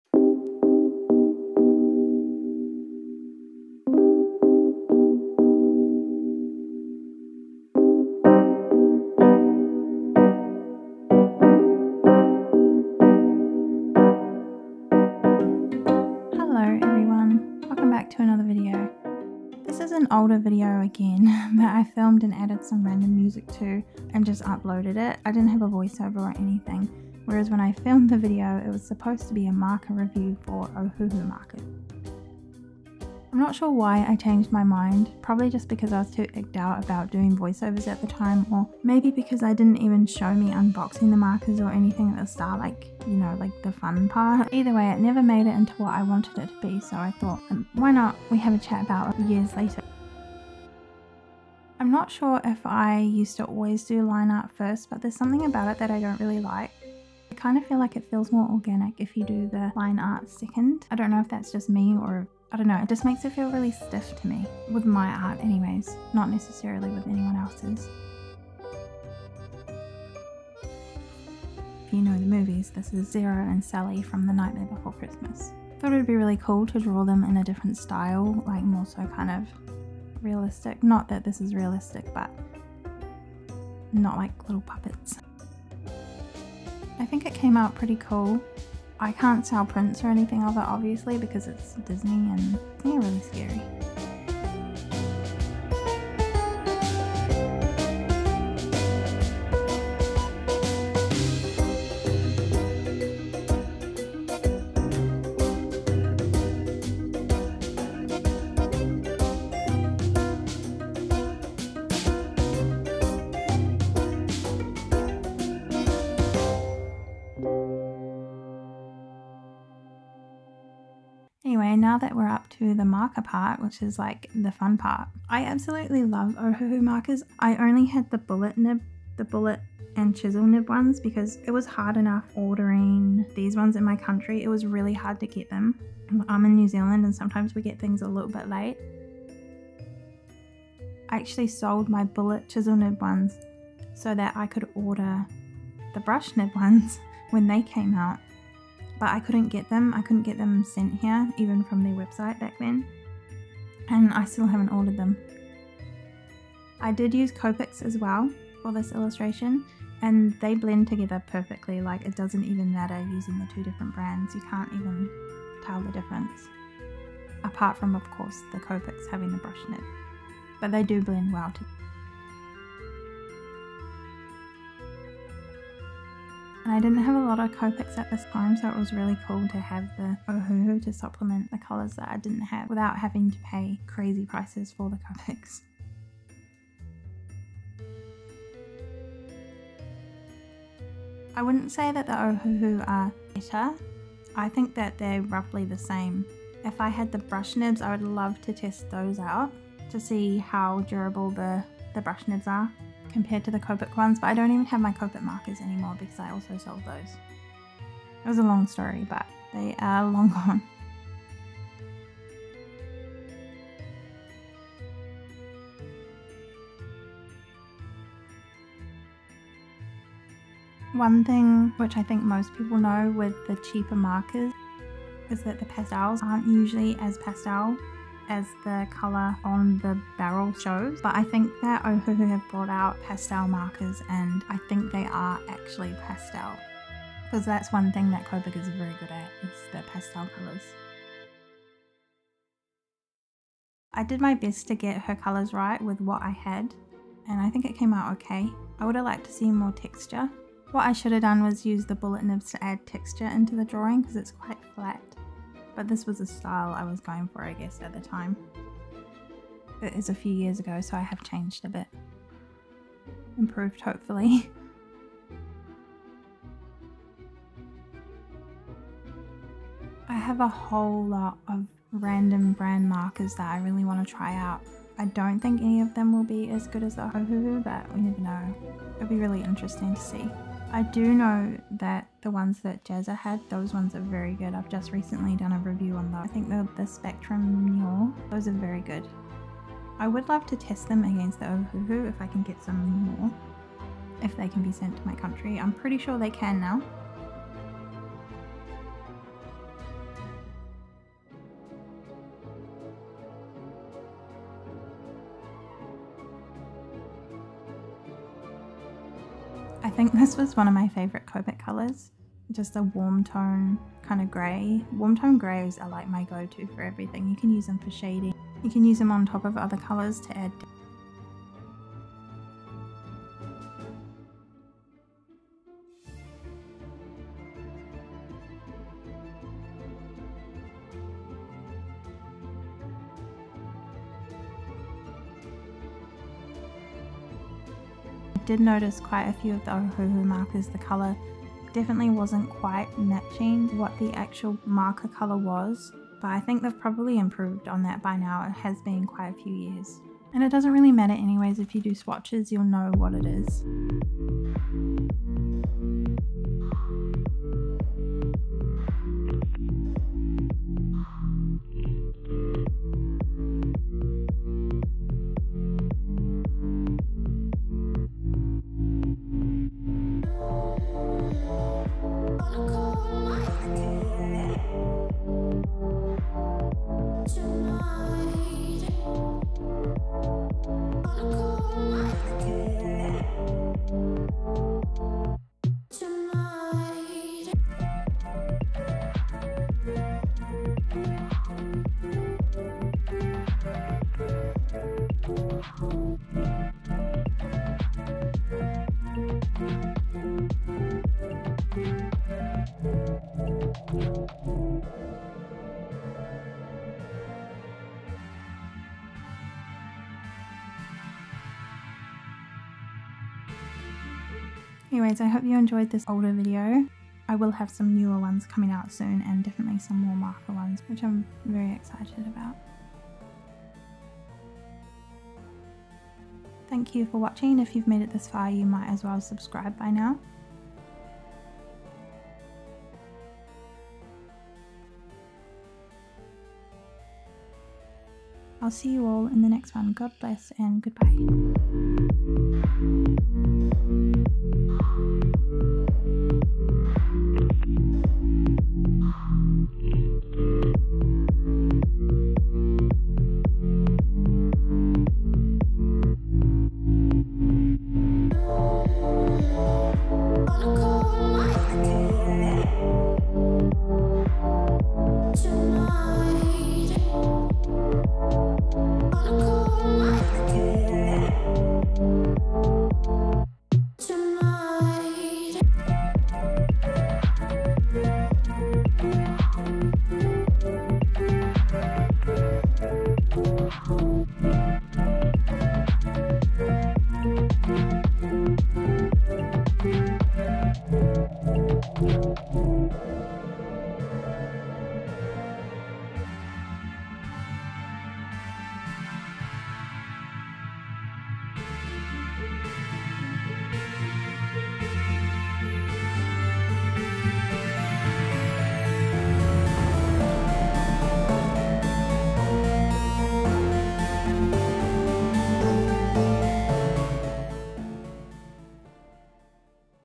Cheap Brush Markers - Better Than Copic? Part II
Hello! Join me while I test out Ohuhu markers! I did this a while ago (years) before I knew much about anything to do with filming and the video is a little weak compared to what my newer marker reviews will be so stick around for those :D